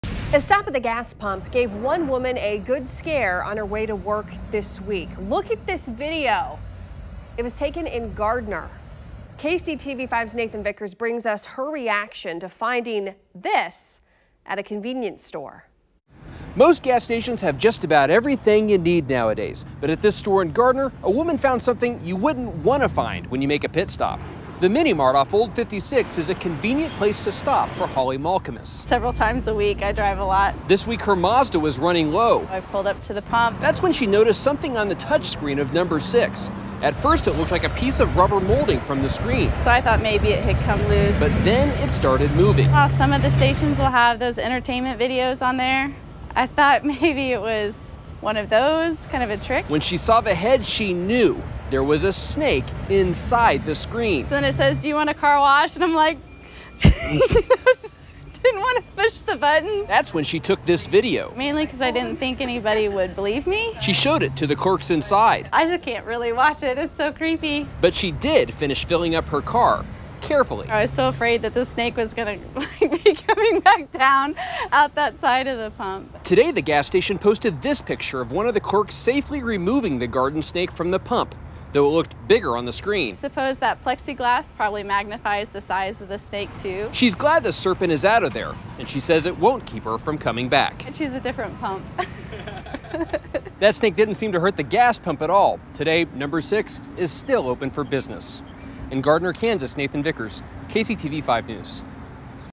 Woman spots snake in gas pump’s screen in Gardner
A stop at a gas pump gave one woman a good scare on her way to work.